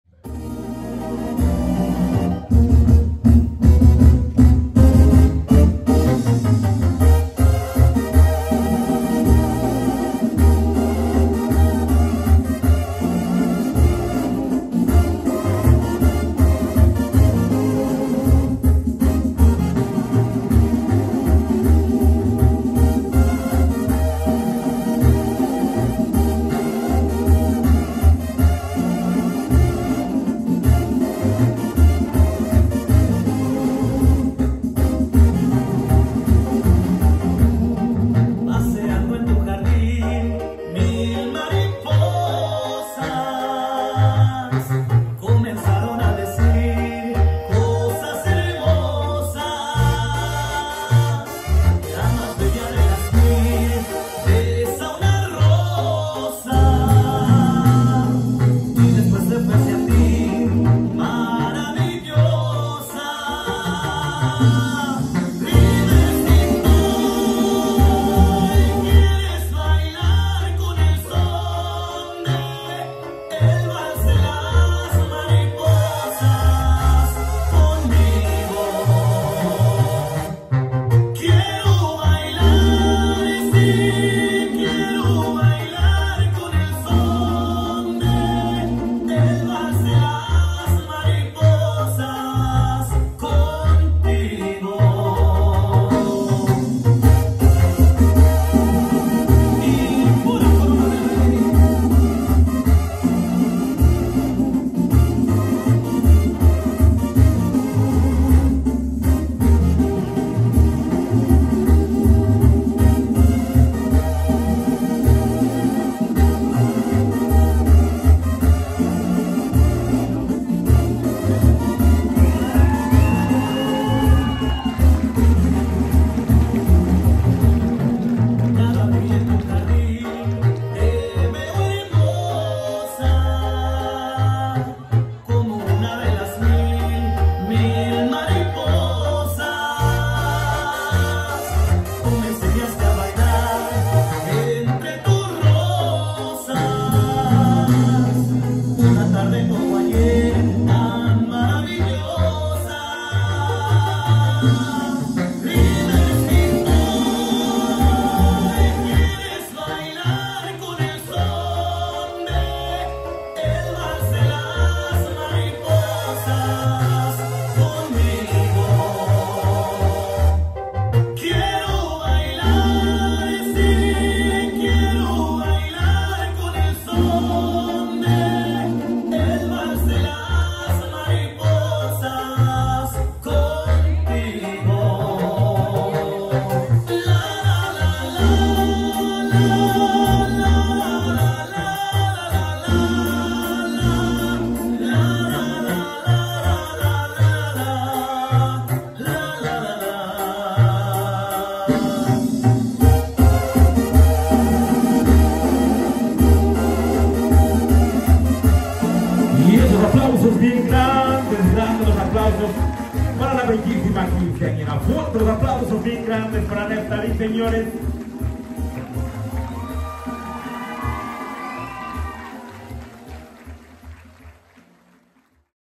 NEFTALÍ RODRÍGUEZ VALS PRINCIPAL
“El brillo de tu alma, te hace una mujer sumamente hermosa por fuera y por dentro”. Felicidades Neftalí por tu fiesta de XV años. 👑💚

Corte de Honor:
Kevin Villanueva
Jonathan Lagunas
Angel Olan
Fabián Ledezma
Iván Villanueva
Antony Duarte
Alan Contreras
Jakob Zárate

Horacio Cabrales
Coreógrafo & MC
Quinceañeras Dance Studio
Dance Studio: 3701 W 63rd ST. Chicago IL